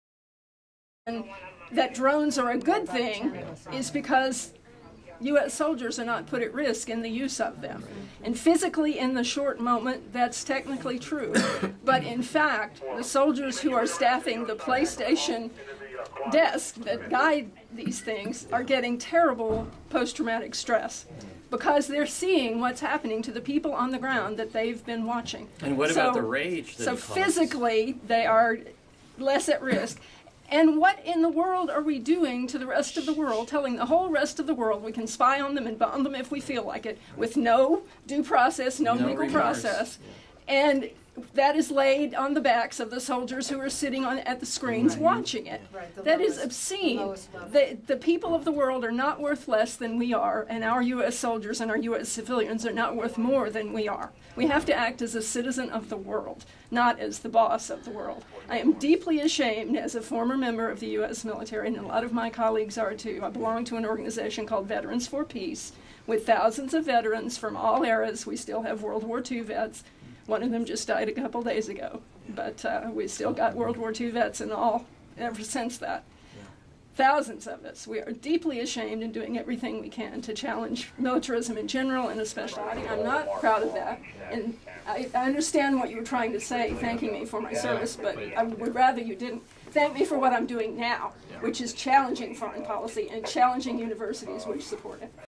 Drones sit in @ Johns Hopkins U with Ellen Barfield
Anti Drone activist occupy the Dean's office of John Hopkins U to protest the research done @ the applied physics lab